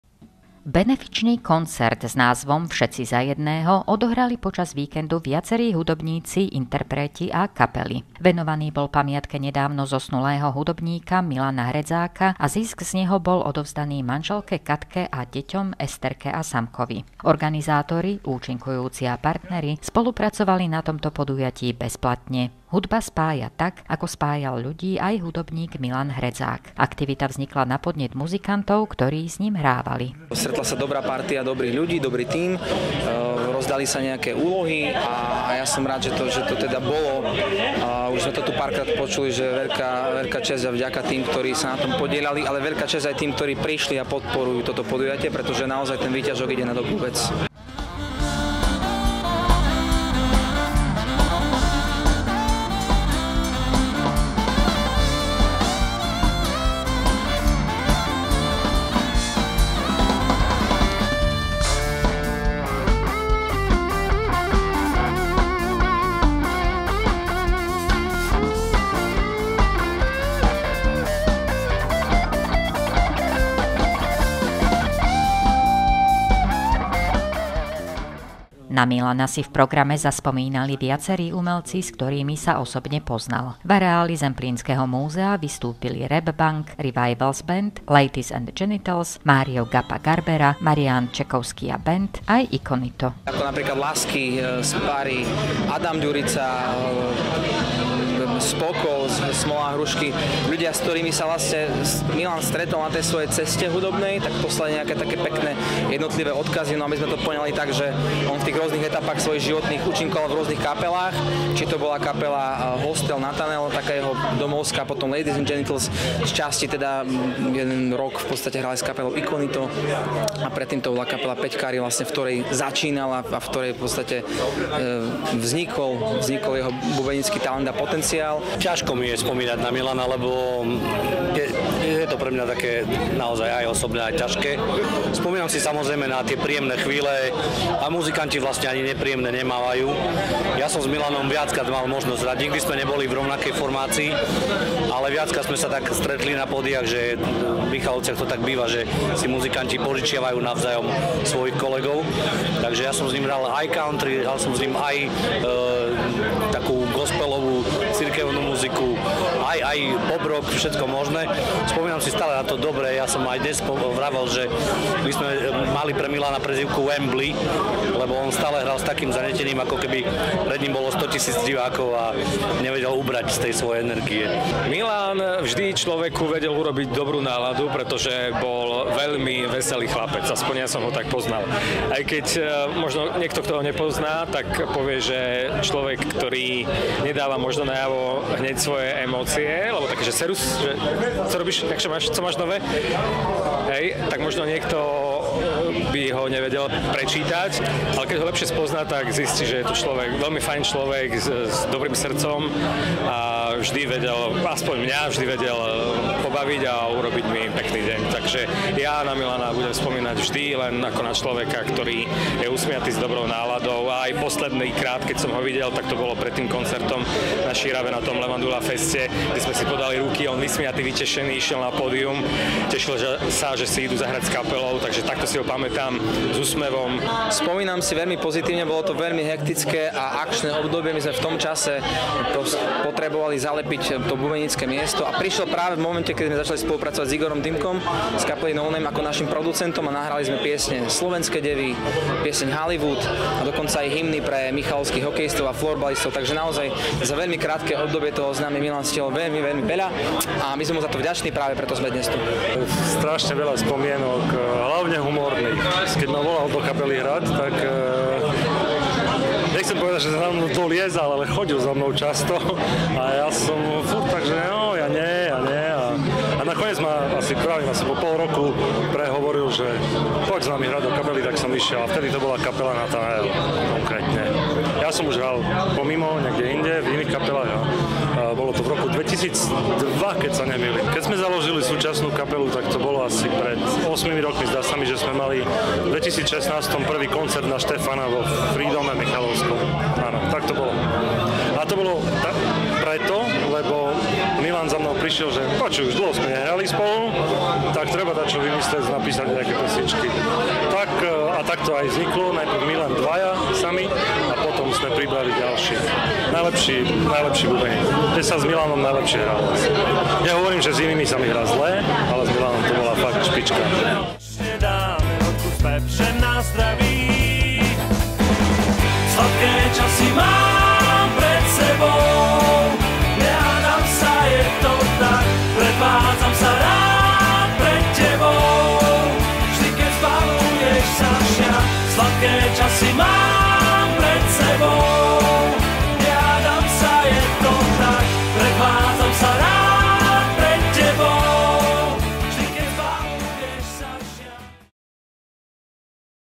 (Všetci za jedného) Benefičný koncert pre Milana Hredzáka
Dnes uzrela svetlo sveta reportáž TV Mistral o benefičnom podujatí “Všetci za jedného”.
Ďakujeme za Vašu účasť a mobilizáciu pre dobrú vec! Ďakujeme aj tam hore tebe Milanko za stopu, ktorú si v nás zanechal!
Pre tých, ktorí sa nemohli zúčastniť a radi by pomohli bol zriadený transparentný účet - nájdete ho na
Ďakujeme, že pomáhate!